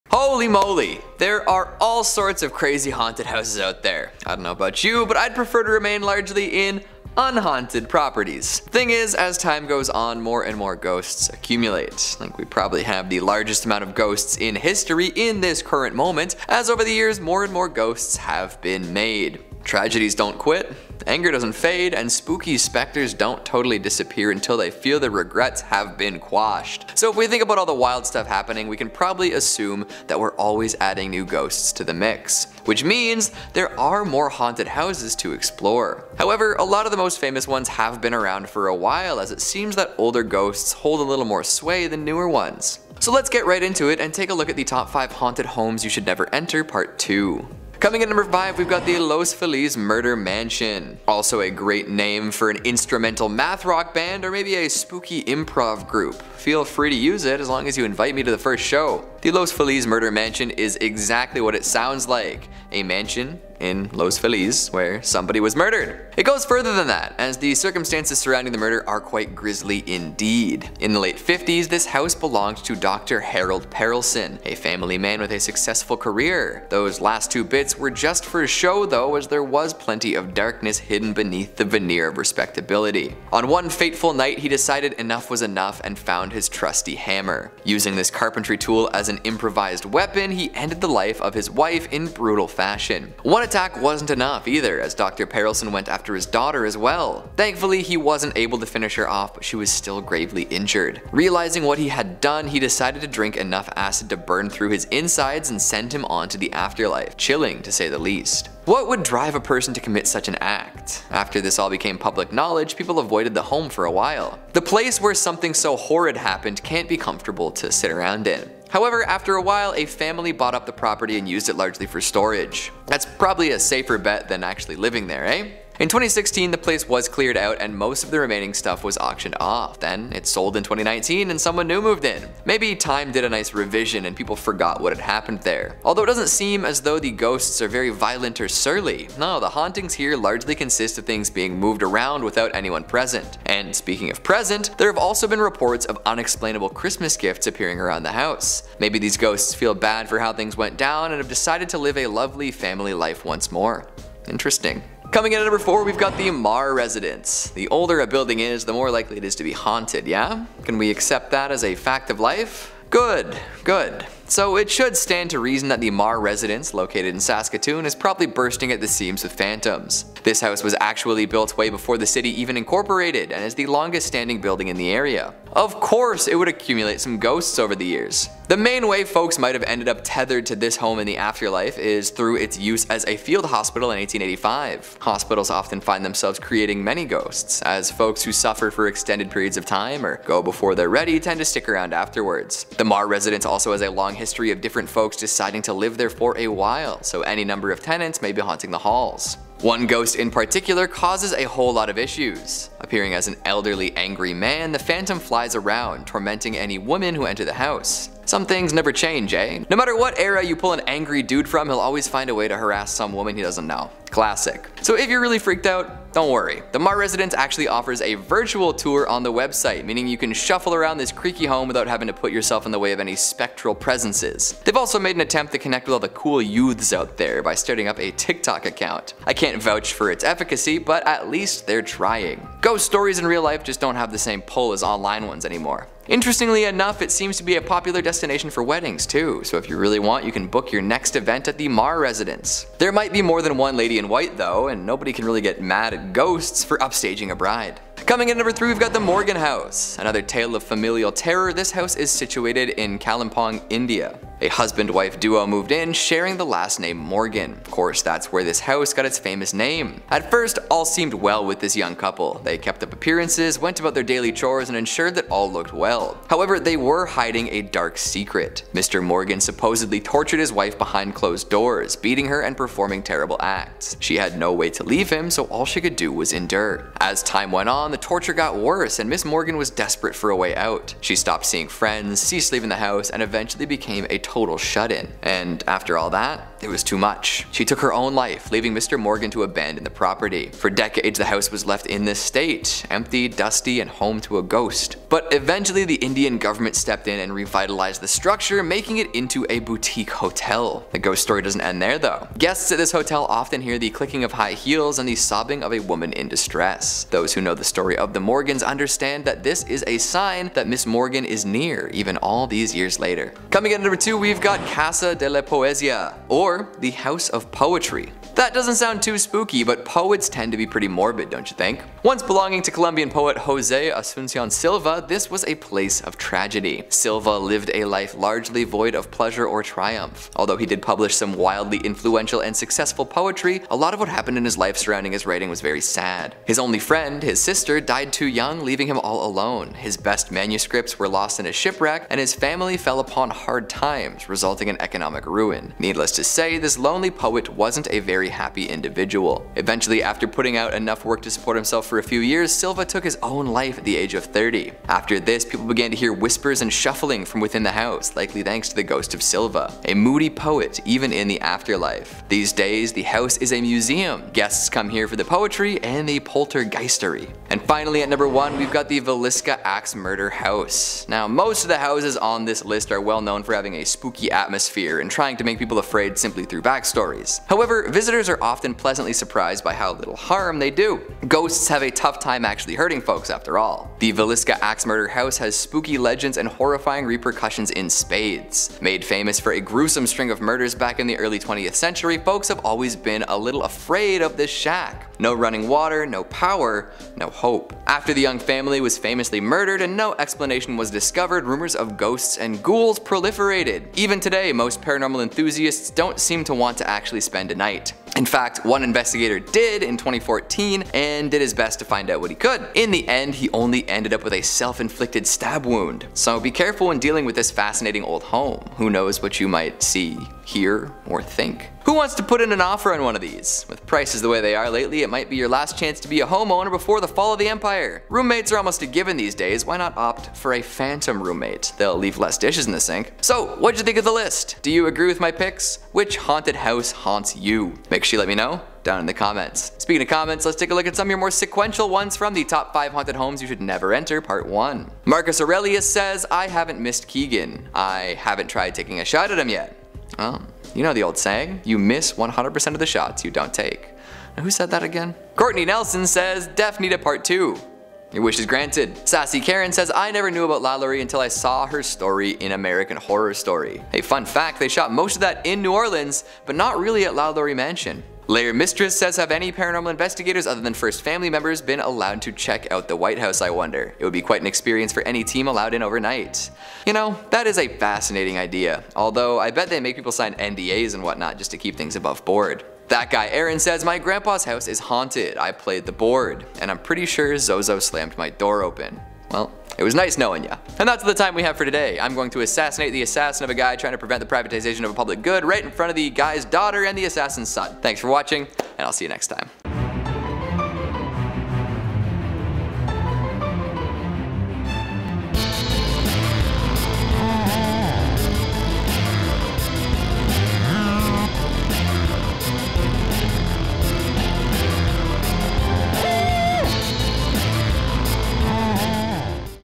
Top 5 Haunted Homes You Should Never Enter - Part 2
Haunted Houses seem cute until it is your own scary haunted home. Ghosts and spirits are found lingering in these homes and we have all of the spooky stories. Some have danger, scary stories, or unexplained mysteries.
Stay tuned for a piece of the paranormal investigation as we are back with the next installment in this series: Top 5 Haunted Homes You Should Never Enter - Part 2 only here on Top 5 Scary Videos!

Time Codes:
0:00 - Intro
0:47 - Los Feliz Mansion
2:35 - The Marr Residence
4:08 - Morgan House
5:13 - Casa De La Poesia
6:07 - Villisca Axe House
7:13 - Outro & Comments